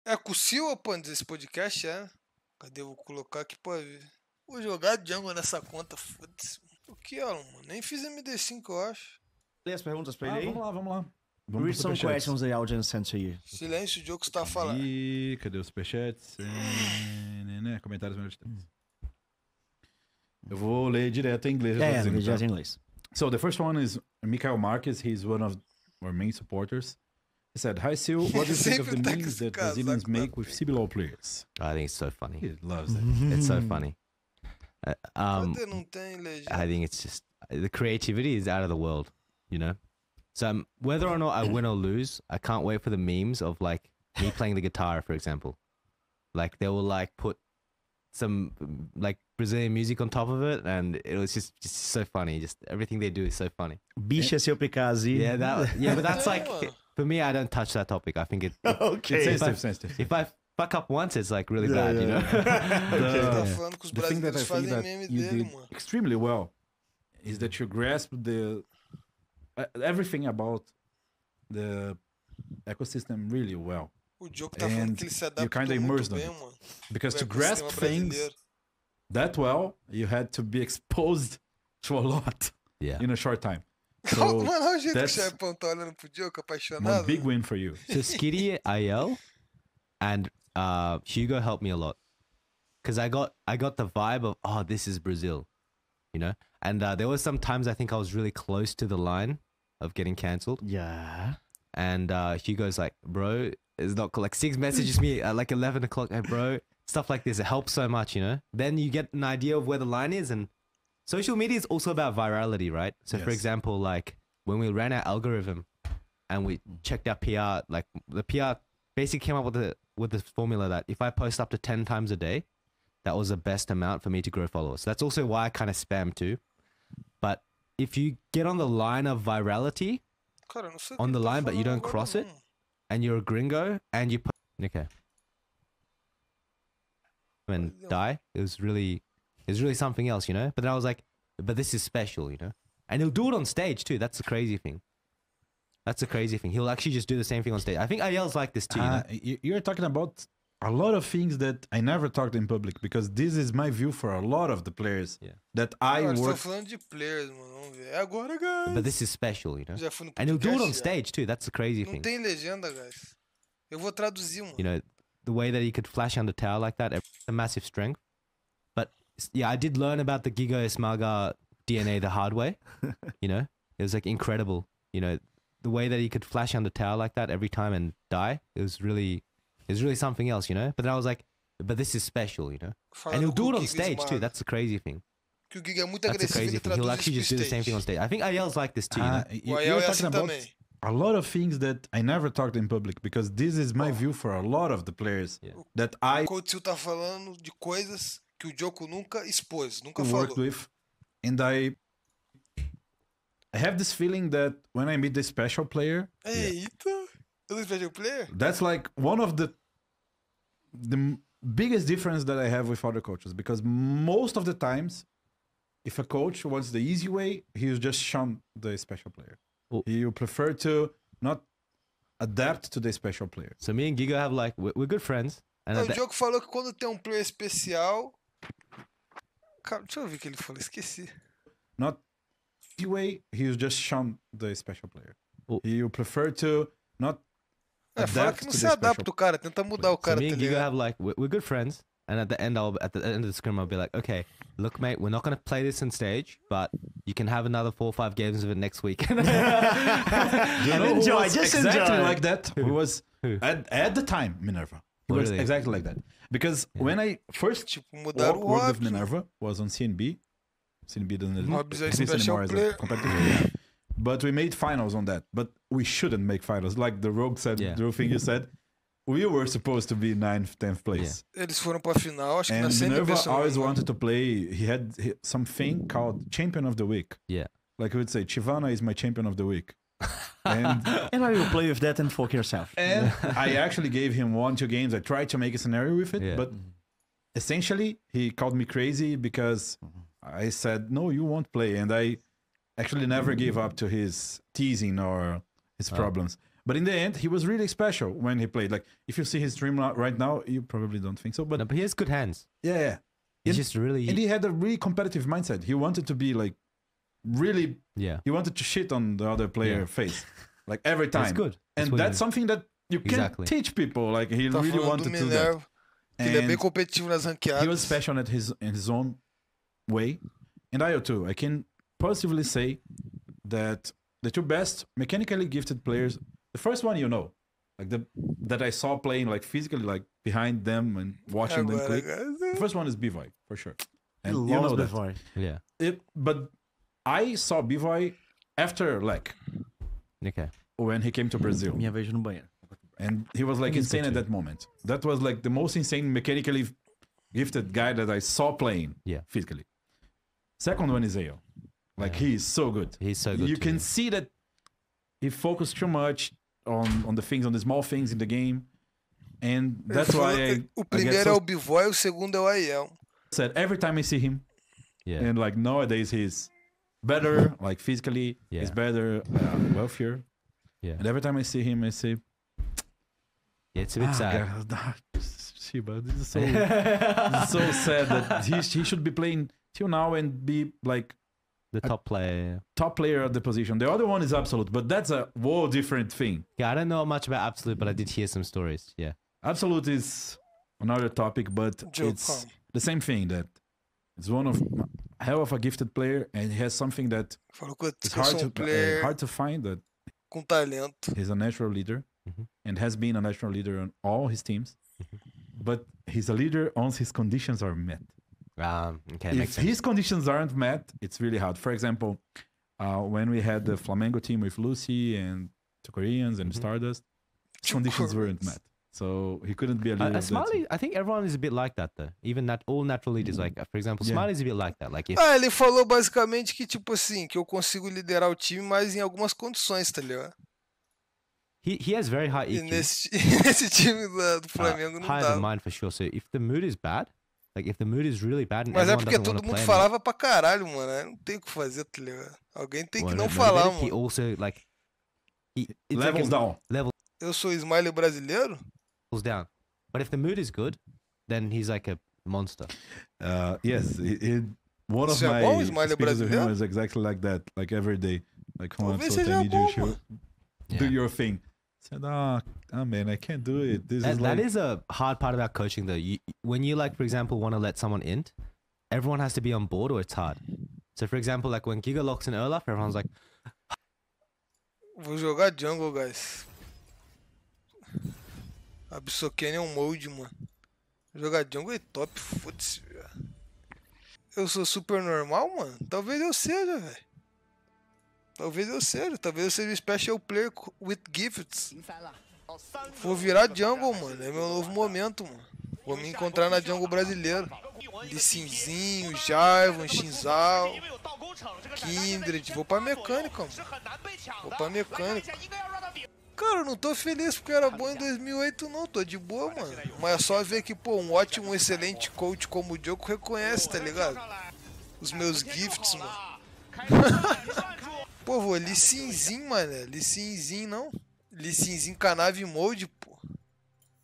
DJOKO EMOCIONANDO o ABSOLUT ao FALAR QUE ELE ERA um EXCELENTE PLAYER de LOL | Absolut React
✂️ CORTES das LIVES do ABSOLUT

💜 Live todos os dias (ou quase) na roxinha!

🎬 Edição e Thumb: @oykaD aka Dakyo